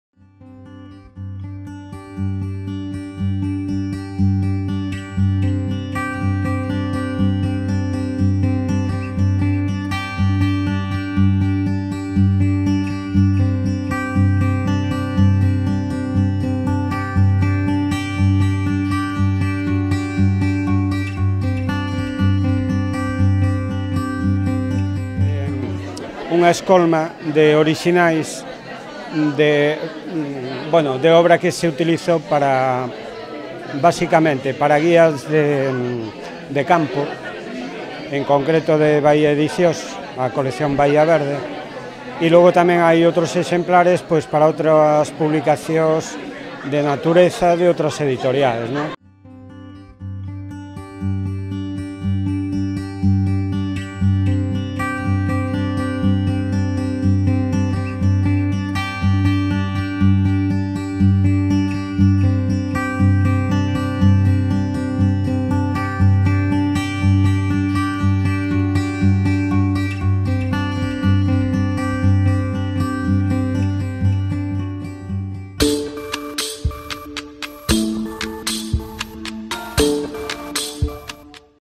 Arranca o III Encontro "Compostela Ilustrada"
Arrancou o III Encontro Internacional de Cadernos de Viaxe Compostela Ilustrada, que se celebrará ata o día 11, baixo o lema "Natureza urbana". O programa arrancou coa inauguración da exposición Unha ollada natural, con obras de Calros Silvar.

A exposición Unha ollada natural foi inaugurada na Galería Sargadelos. A mostra é unha pequena escolma do traballo que leva realizando Calros Silvar para guías de natureza, con obras nas que se presentan pinceladas da biodiversidade de Galicia: aves, mamíferos, anfibios, fungos, plantas e árbores